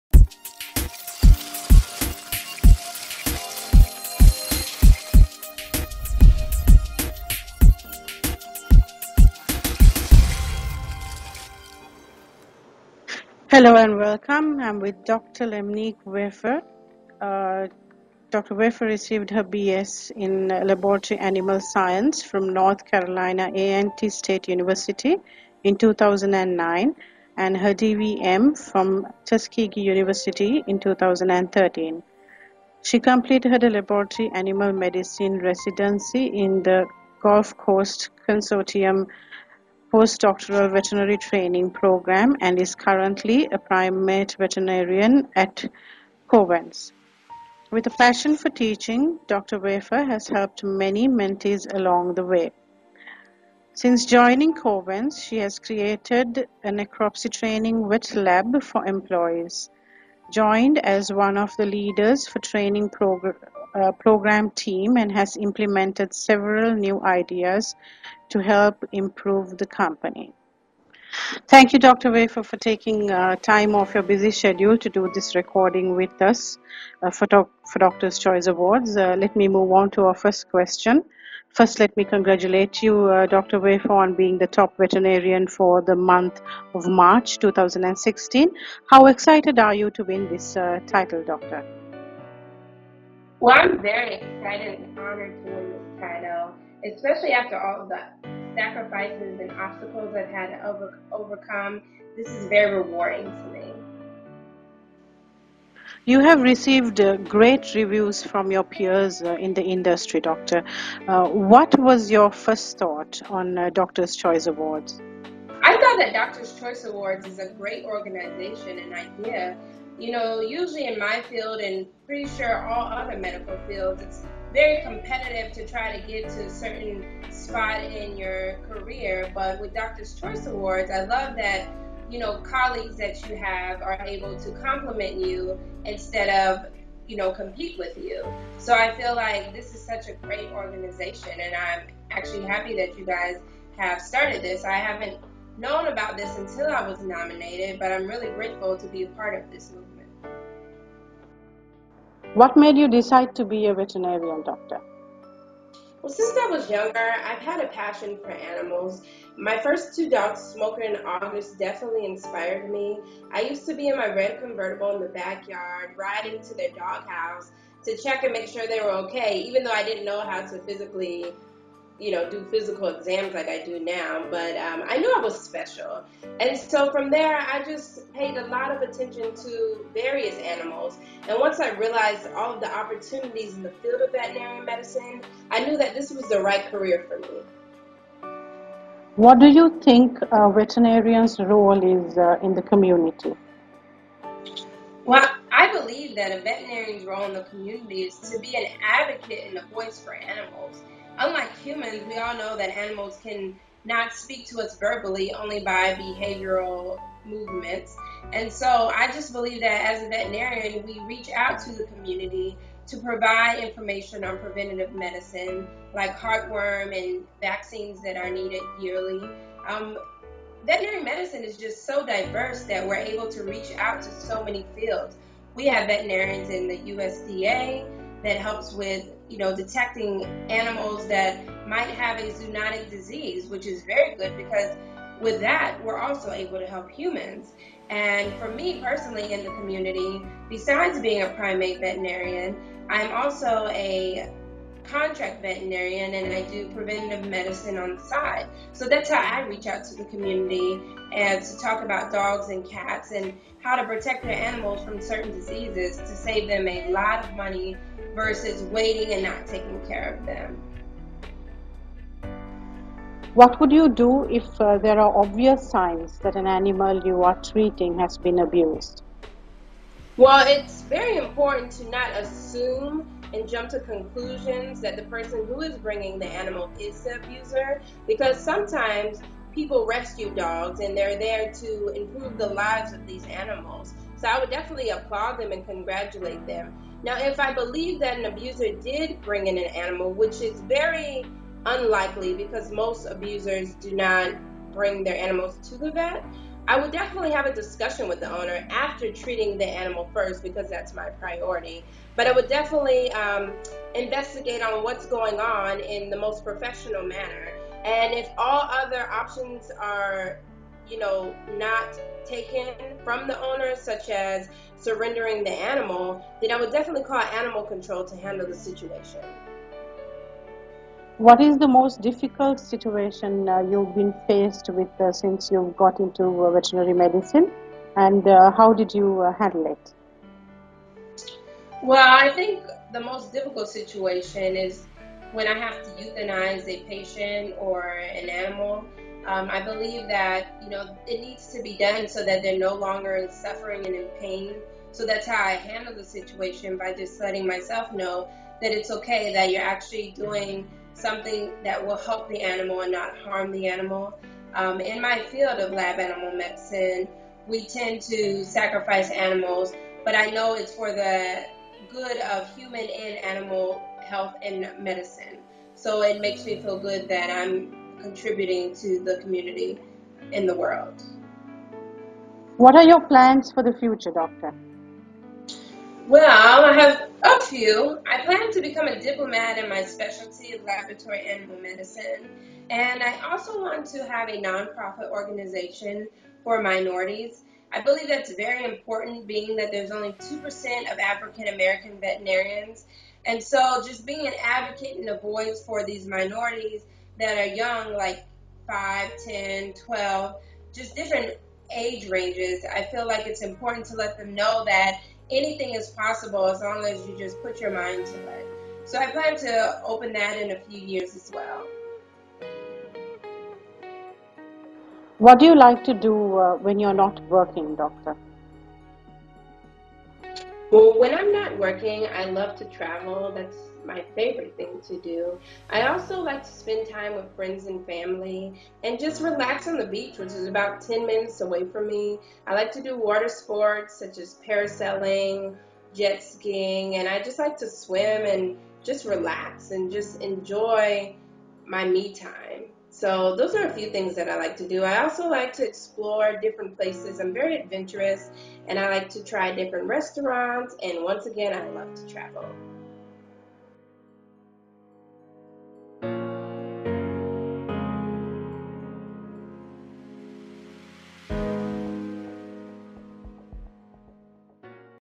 Top Veterinarian for March 2016 – Dr. Lemnique Wafer shares her experience
Dr. Lemnique Wafer received her BS in Laboratory Animal Science from North Carolina A&T State University in 2009 and her DVM from Tuskegee University in 2013. She completed her laboratory animal medicine residency from the Gulf Coast Consortium Postdoctoral Veterinary Training Program and is currently a primate veterinarian at Covance. WIth a passion for teaching, Dr. Wafer has helped many mentees along the way. Since joining Covance, she has created a necropsy training wet-lab for employees, joined as one of the leaders for training program team and has implemented several new ideas to help improve the company.